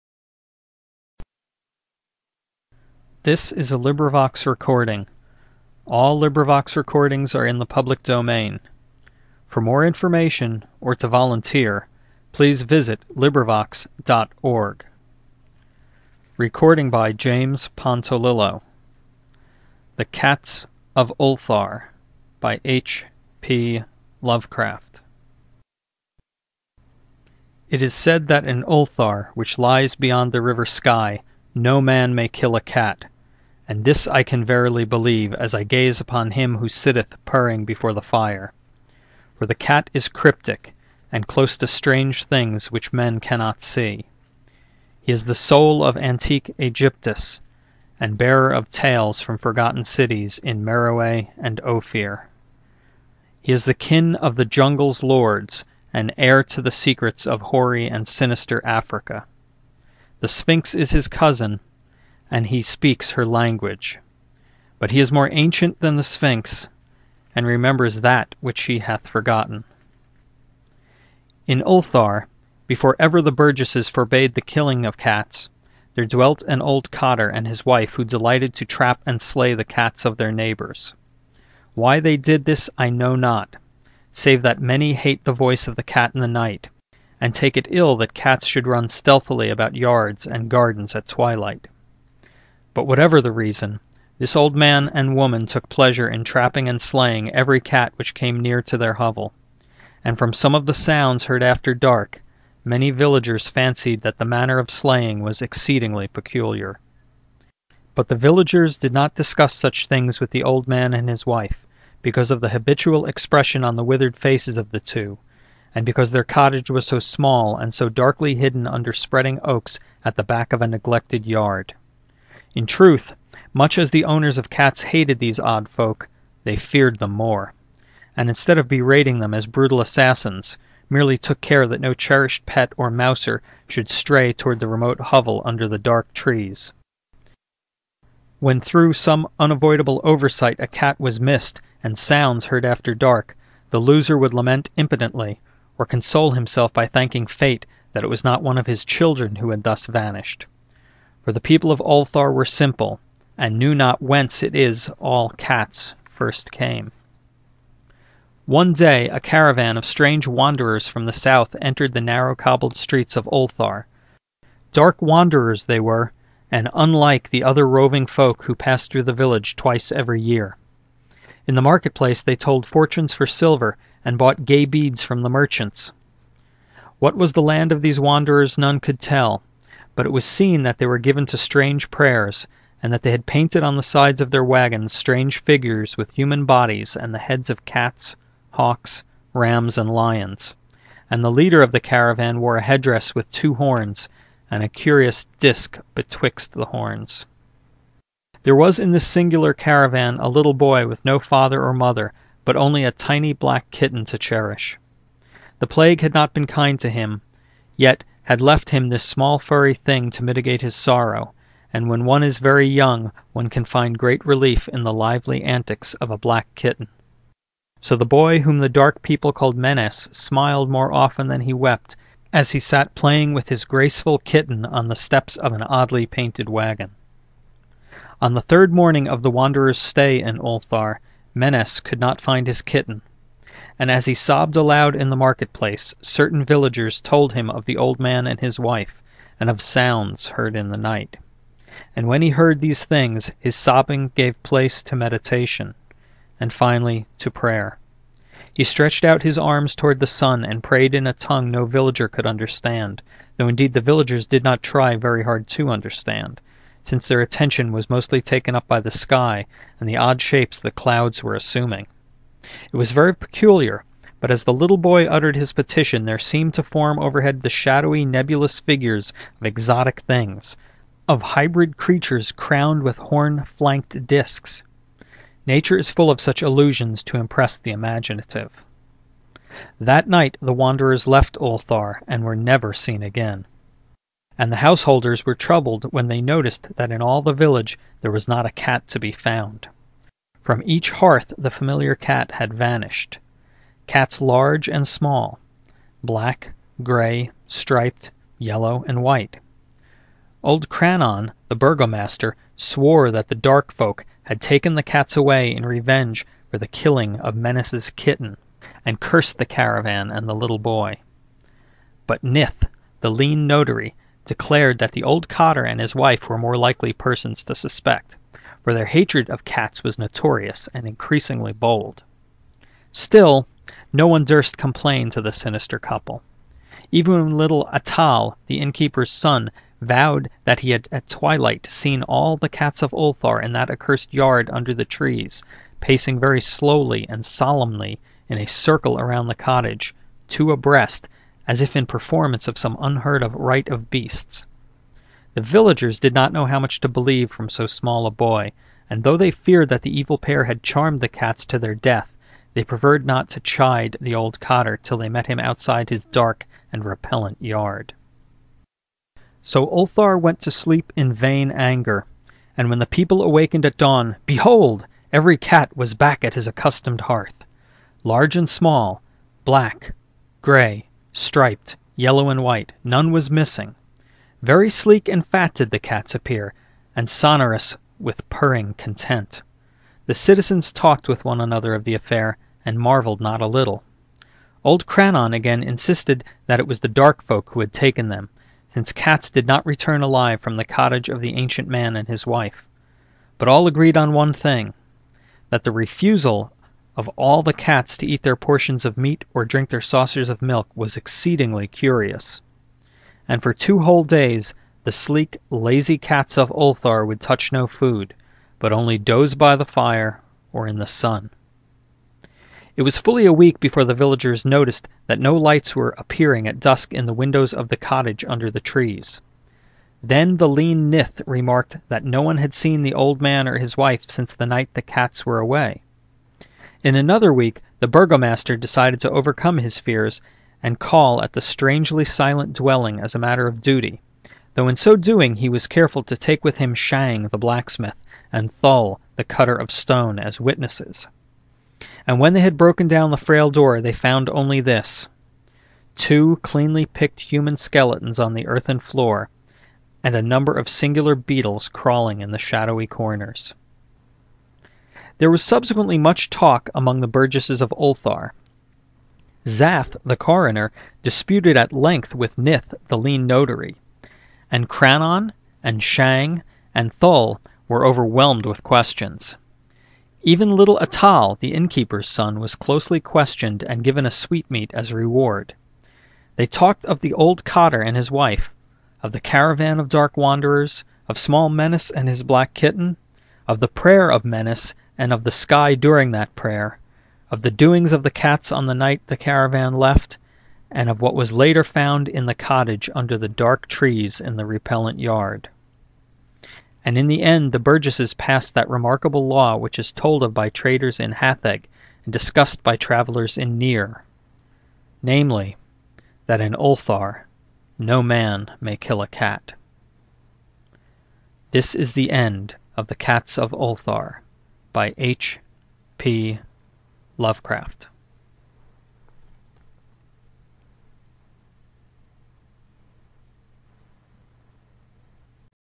The Cats of Ulthar
Author: H.P.Lovecraft
Reader:jpontoli

Script
シナリオ読込型VRoidビューワ HachiViewerPlus